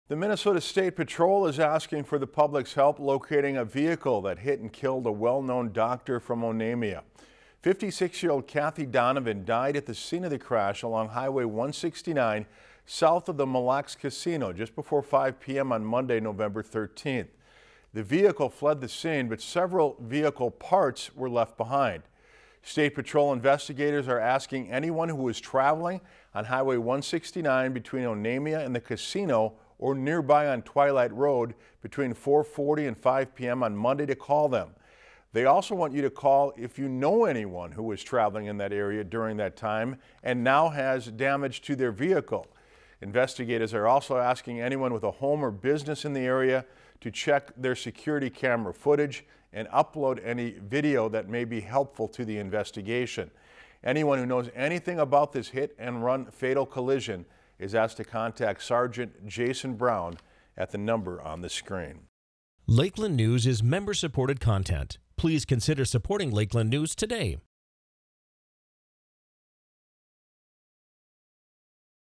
MN State Patrol Asking for Help Finding Vehicle in Fatal Onamia Hit-and-Run | Lakeland News
Originally aired November 16, 2023 on Lakeland News